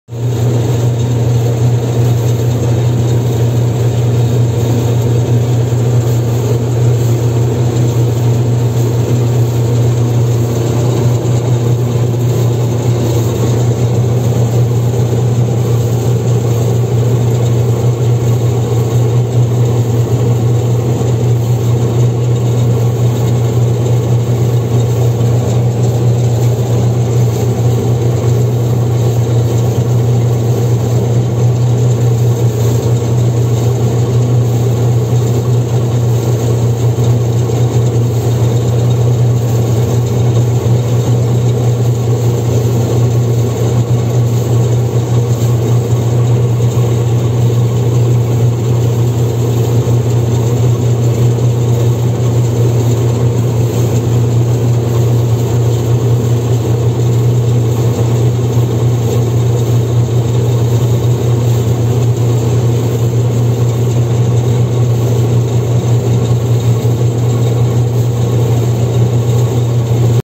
Massey Ferguson 16 widok z kabiny, żniwa 2022
Żniwa 2022 w kabinie kombajnu Massey Ferguson 16